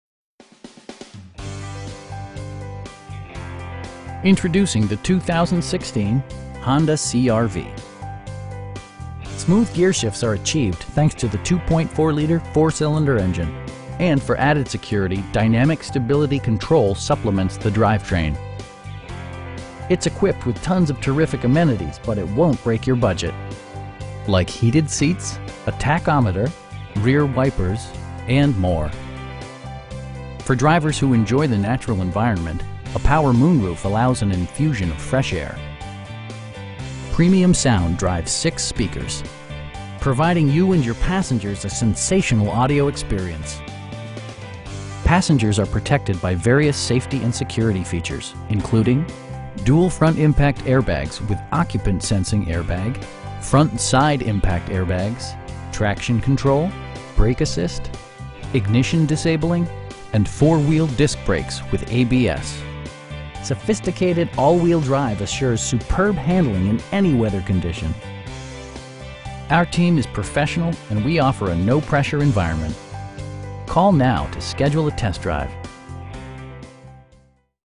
2016 Honda CR-V AWD 5dr EX
Huggins Honda
7551 NE Loop 820

Introducing the 2016 Honda CR-V. Smooth gearshifts are achieved thanks to the 2.4 liter 4 cylinder engine, and for added security, dynamic Stability Control supplements the drivetrain. It's equipped with tons of terrific amenities, but it won't break your budget. Like heated seats, a tachometer, front bucket seats, air conditioning, power door mirrors, cruise control, rear wipers, and more. For drivers who enjoy the natural environment, a power moon roof allows an infusion of fresh air. Premium sound drives 6 speakers, providing you and your passengers a sensational audio experience. Passengers are protected by various safety and security features, including: dual front impact airbags with occupant sensing airbag, front SIDE impact airbags, traction control, brake assist, ignition disabling, and 4 wheel disc brakes with ABS. Sophisticated all wheel drive assures superb handling in any weather condition. Our team is professional, and we offer a no-pressure environment. We'd be happy to answer any questions that you may have.